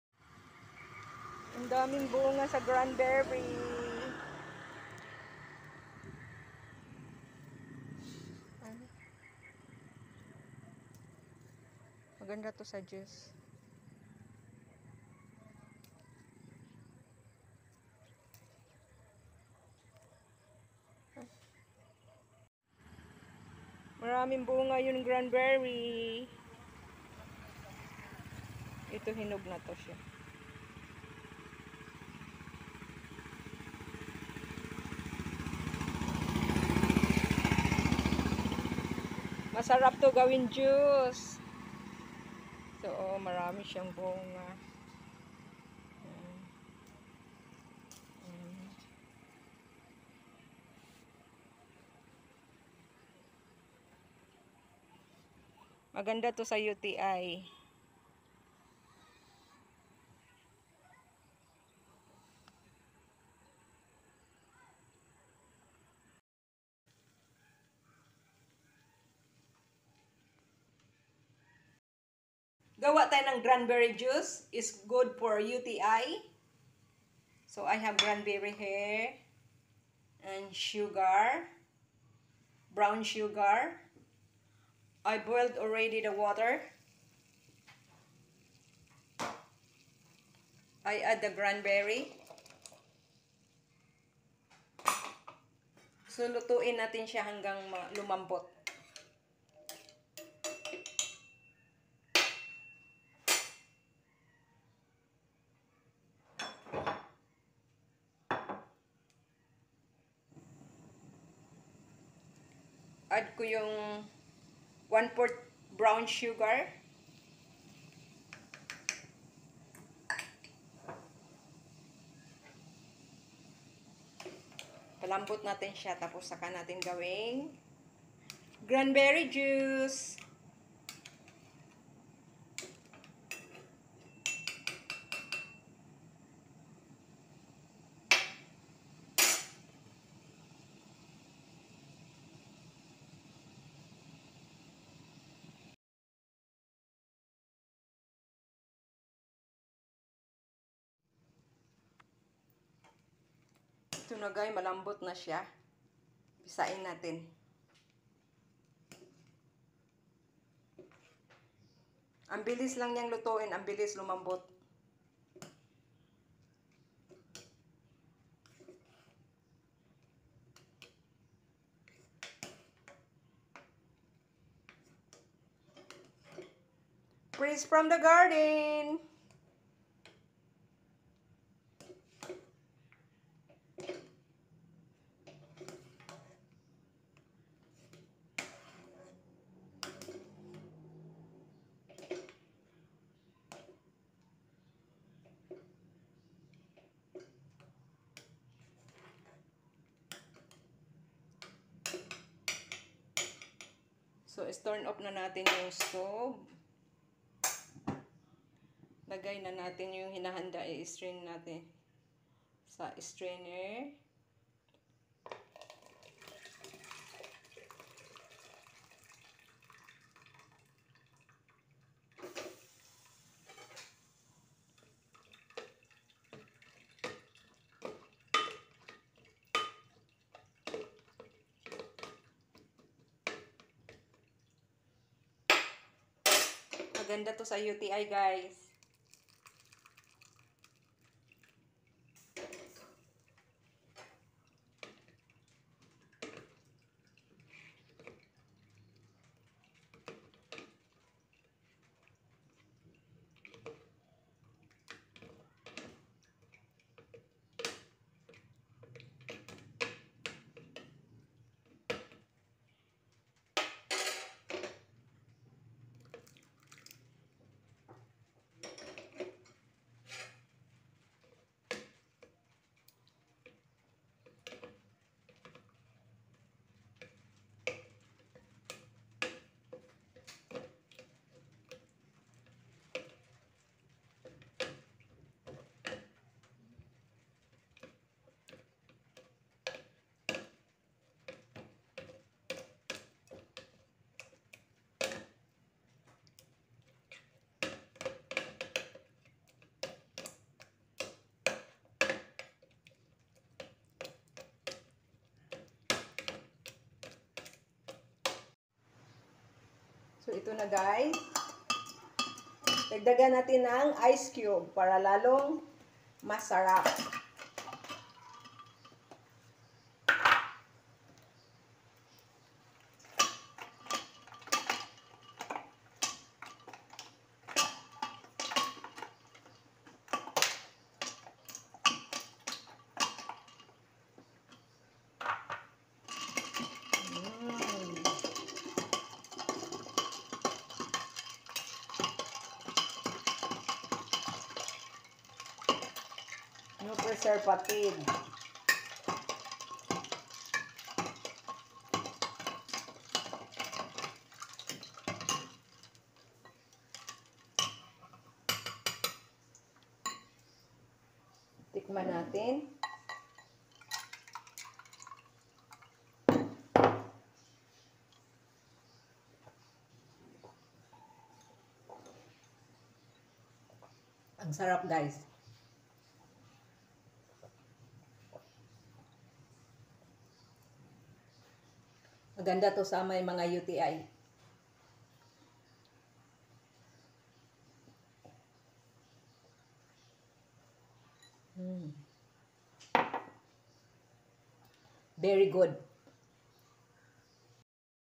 Home Made Fresh Cranberry Juice, Cure UTI And 13 Health Benefits.😍💞💝
Created by InShot Cranberry Juice good to our body to remove bacteria, prevent UTI, Bladder, Kidney Infection and High Vitamin C.